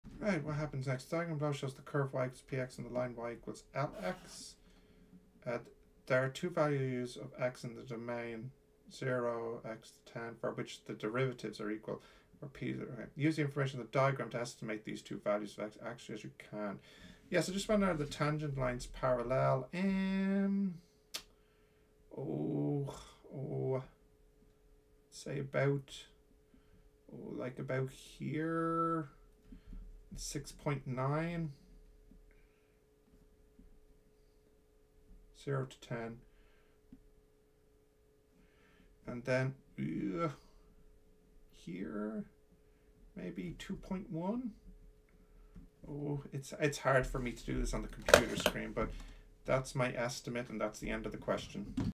leaving cert maths 2024 higher level paper 1 question 3 c (my first attempt)
⚠©Copyright Stephen Easley-Walsh. Ideally if I were a student I would at least read the questions twice HAHA :D but I wanted to rush through it. Spot a mistake? Let me know! Hope it helps.